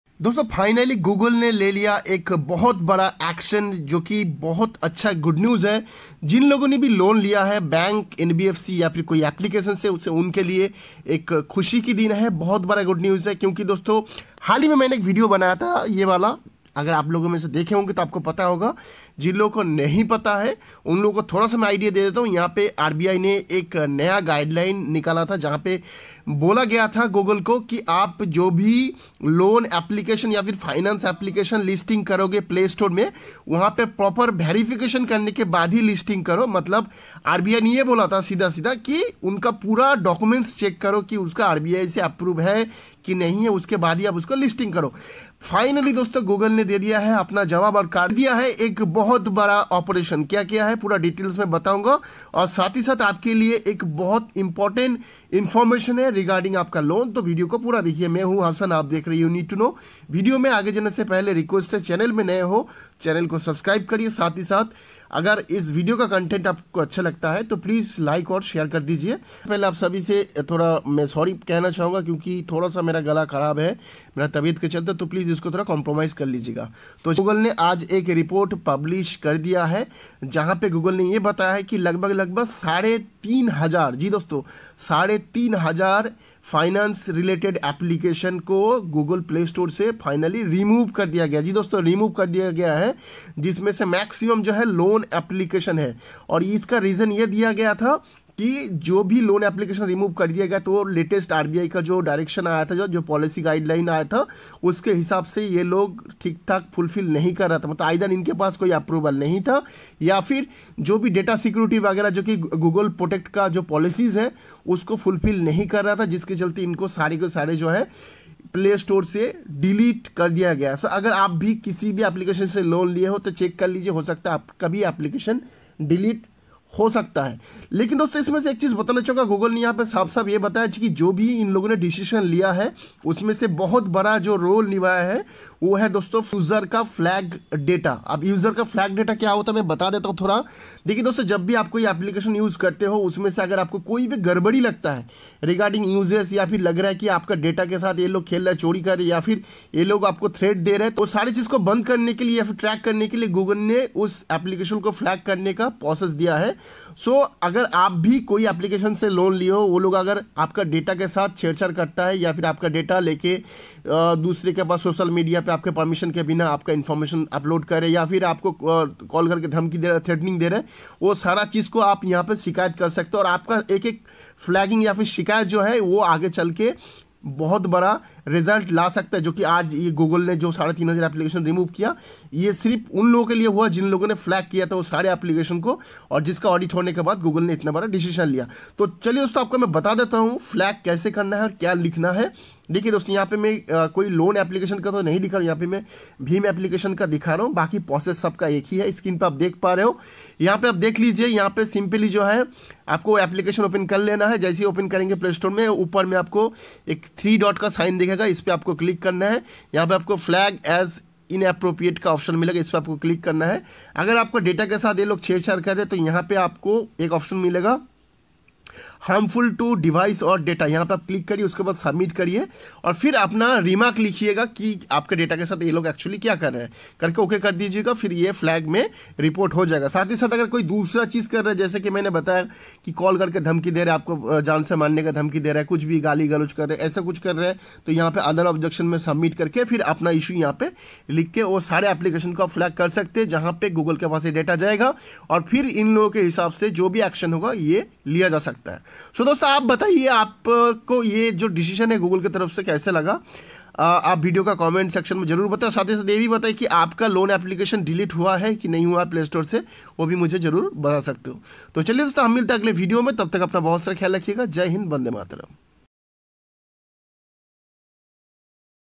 Google Implements RBI Policy for Loan Holders, Offering Big Relief to Borrowers
Google had taken big action as per rbi policy it's great news for loan Holders.